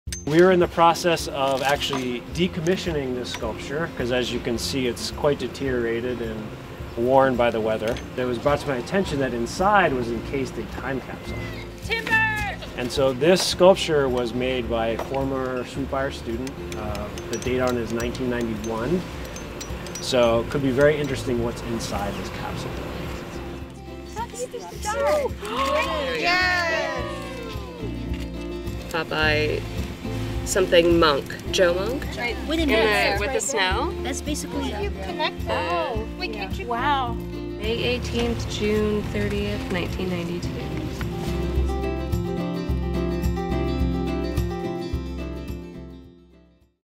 Finding a Time Capsule in a Sculpture
Students from Shawn O'Connor's Materials Studies/3D Design course and Erin Pitt's The Archaeology of Everyday Life course provided muscle power to decommission a sculpture that held a time capsule of Sweet Briar pictures from the early '90s. Sweet Briar alumna Easton Lindquist '92 made the sculpture in 1991 in a class taught by former ceramics professor Joe Monk.